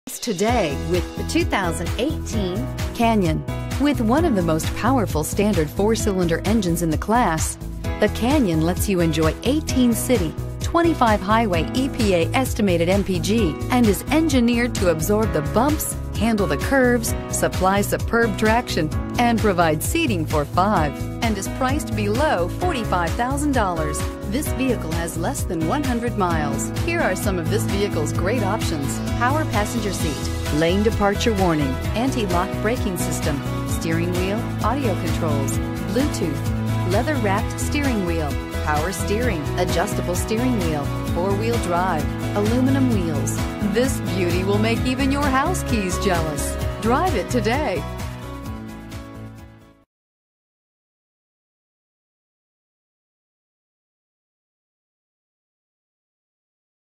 2018 GMC Canyon 18470
2018 GMC CANYON Poughkeepsie, NY
Stock# 18470

2023 South Rd.

Looking for the right car? Today could be your lucky day. With 4 miles, this Satin Steel Metallic, 2018 GMC Canyon equipped with Automatic transmission could be yours. Request more information and set up a test drive right away.